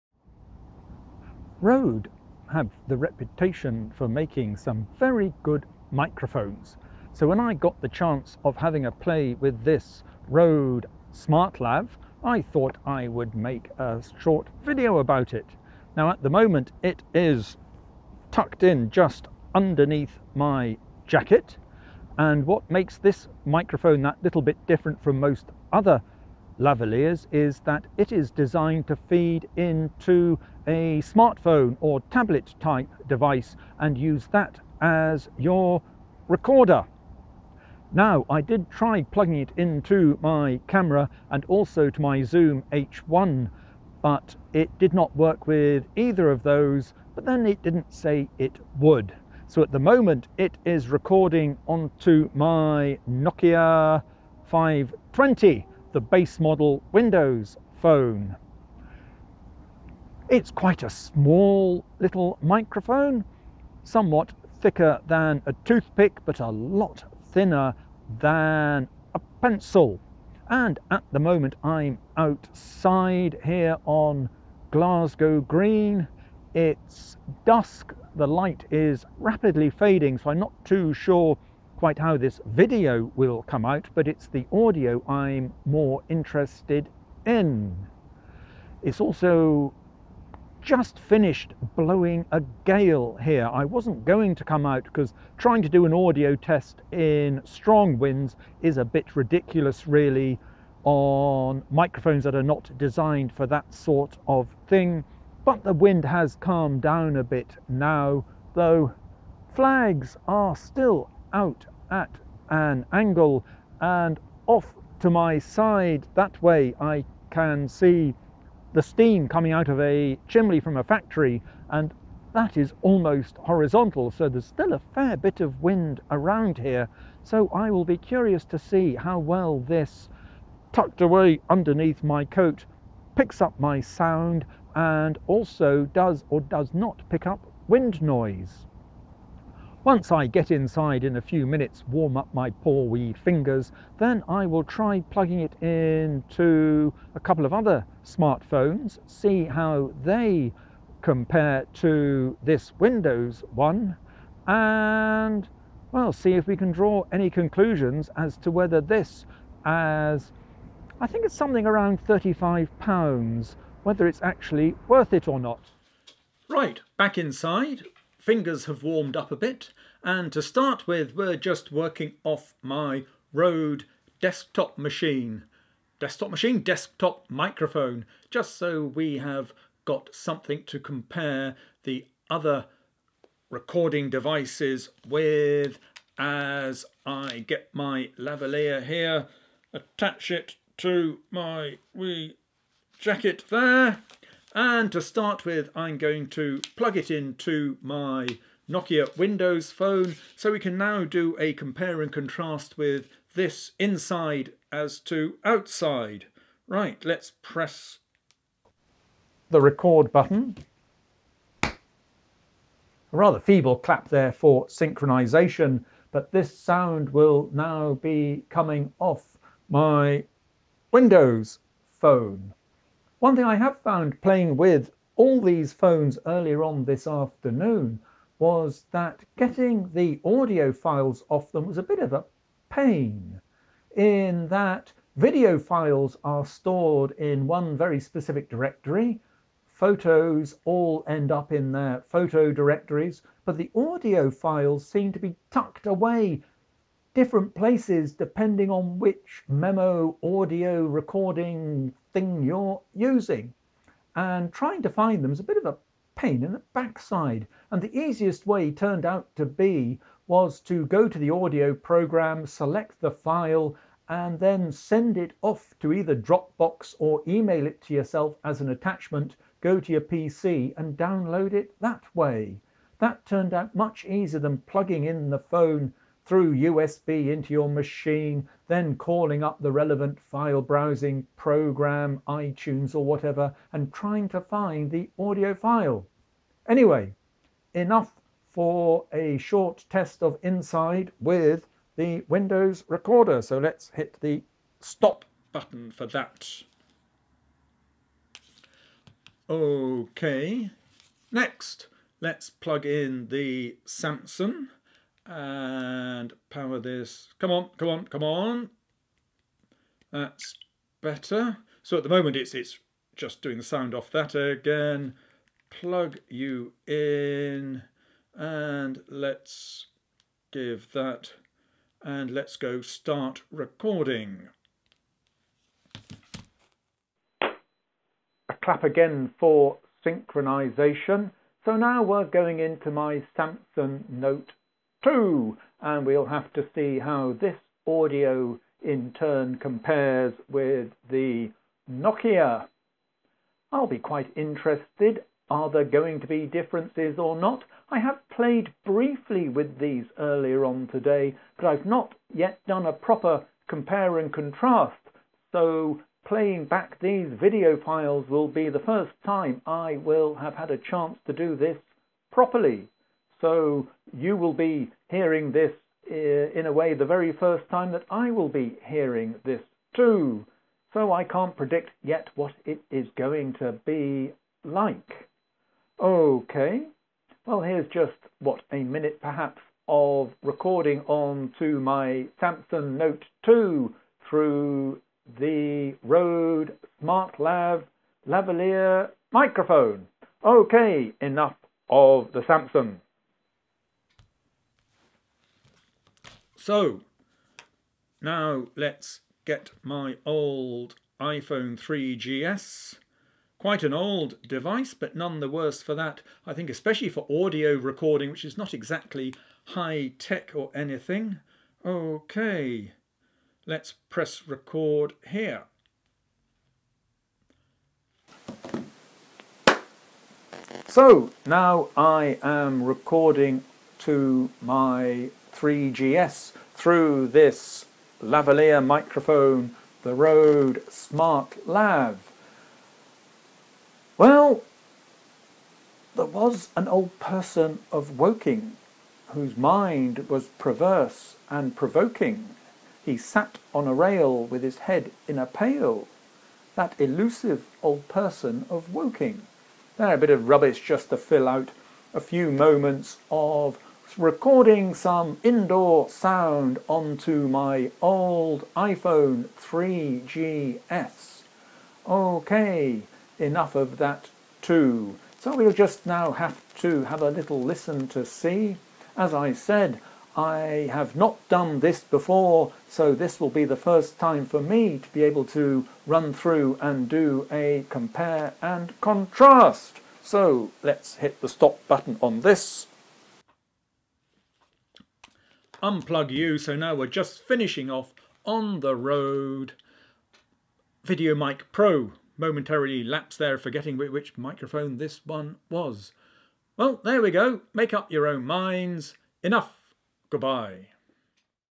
Rode SmartLav microphone test
The Rode SmartLAV is a small lavalier / tie clip microphone designed to work with smartphones.
All tests were done using the phones' default audio recording settings.
Not very impressed.Tried it on a Nokia Lumia 520, Samsung Galaxy Note 2 and iPhone. All gave different results, none of which were particularly impressive.